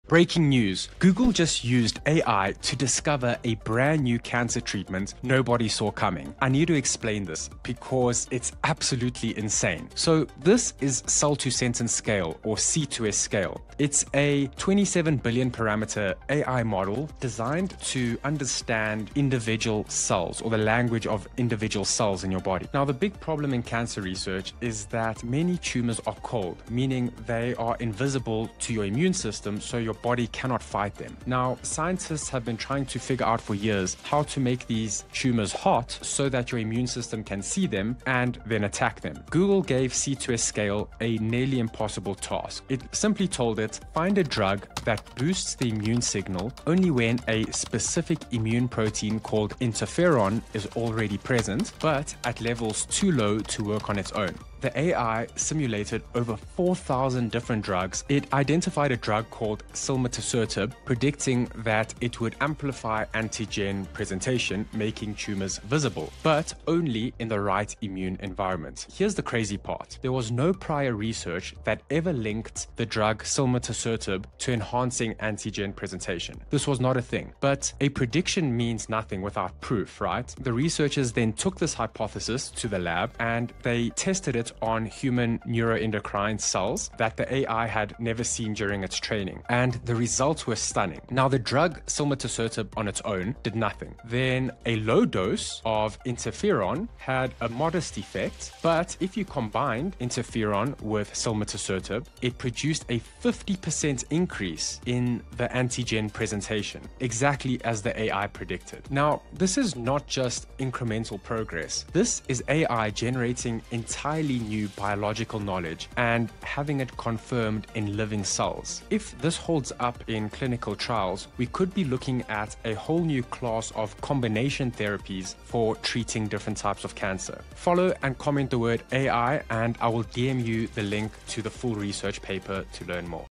How Google is using AI to discover cancer treatments
Google is using AI to discover cancer treatments that previously didn't exist. Comment "AI" and I'll send you the link to the full research paper.